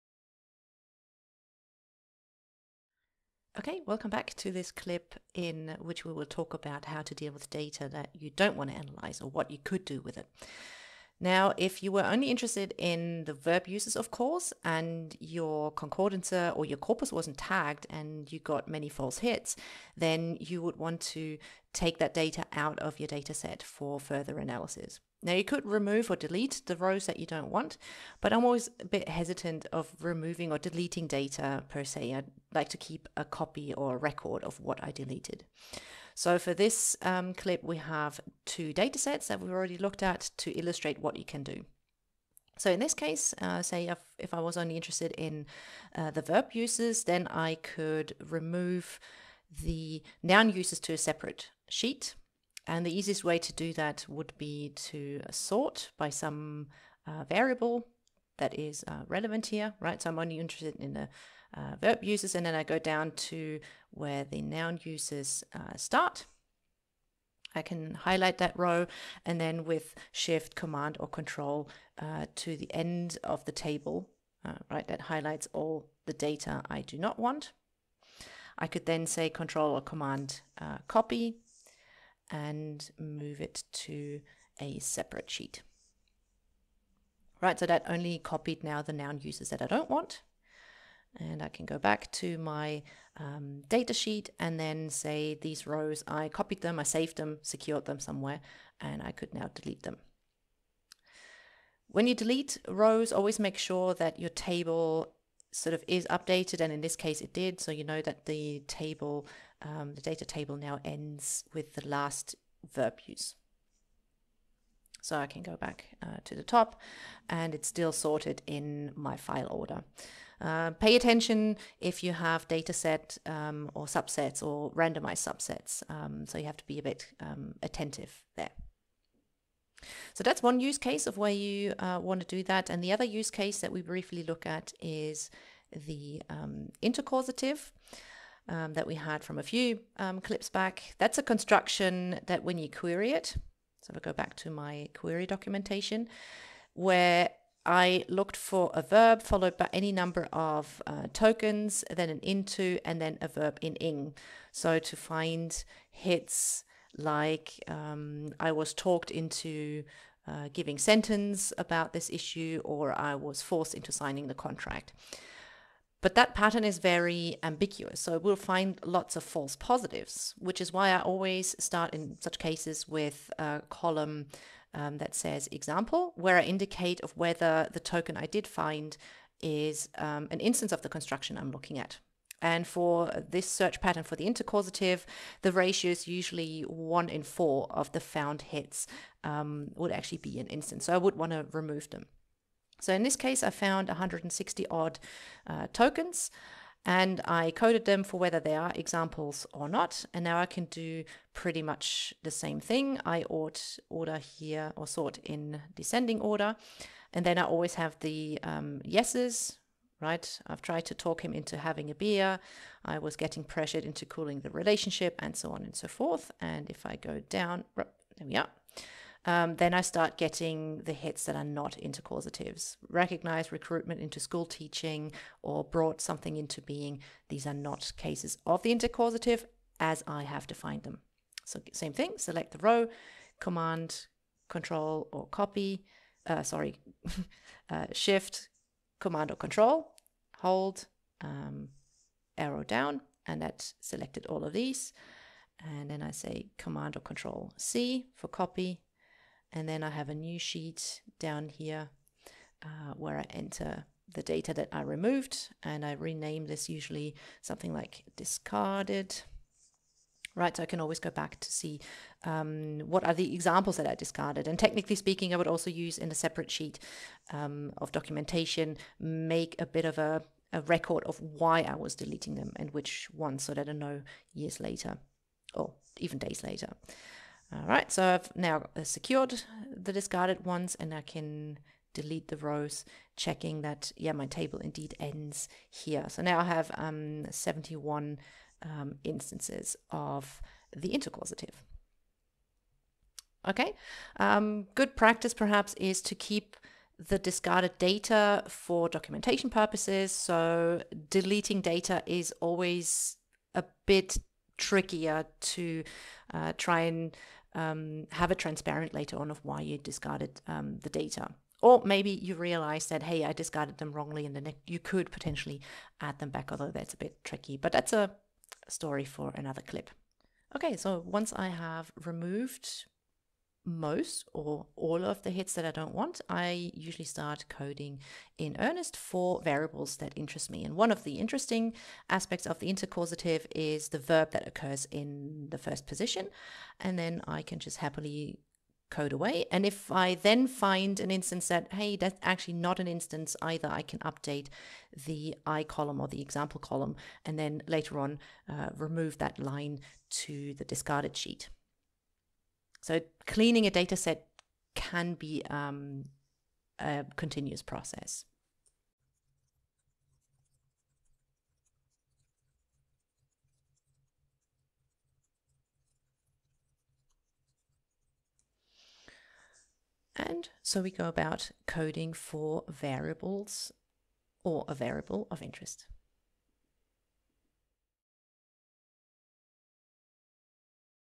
Excel — Annotation 2: False positives
This clip shows how you can deal with false positives: often our corpus query returns more hits than we want, so we need to find a way to deal with that. Deleting lines is one option, but the discarded data should be kept somewhere for documentation purposes. (This is often done manually, especially in queries that are highly structurally ambiguous, as shown in the second half of the clip.)

Leave problems with the clip contents or suggestions for new clips in the columns below.